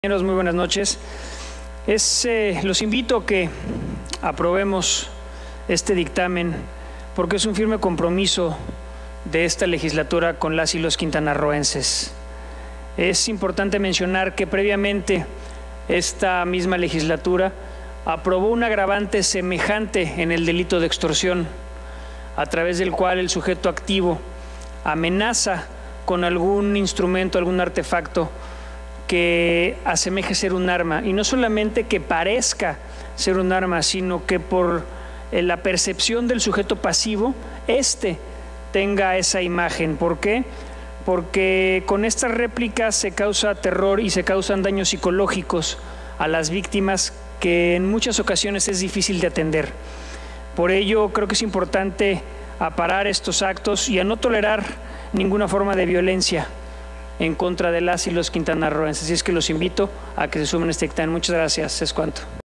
Intervención Dip. Hugo Alday Nieto. PT (02/Octubre/2023)
Intervención del Dip. Hugo Alday Nieto, del PT, en relación al Dictamen con proyecto de Decreto por el que se adiciona la fracción IX del artículo 145 bis del Código Penal para el Estado Libre y Soberano de Quintana Roo; para su aprobación, en su caso. Publicado en la Gaceta Parlamentaria Año II número 07 Ordinaria del 28 de septiembre de 2023.